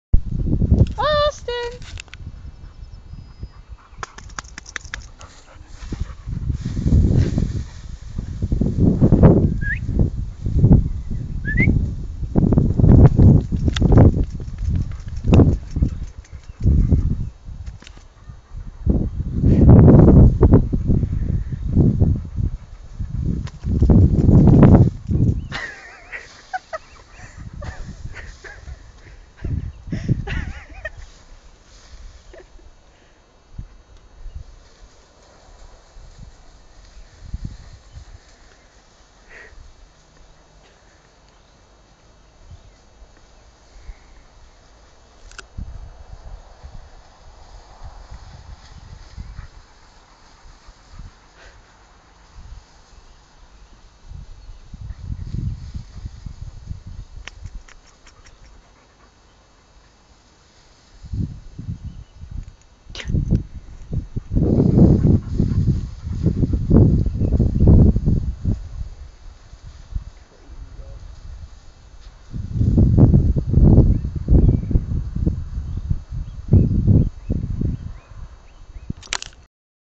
Austin the Kangaroo Dog
This is my dog Austin, who likes to jump in fields like a kangaroo.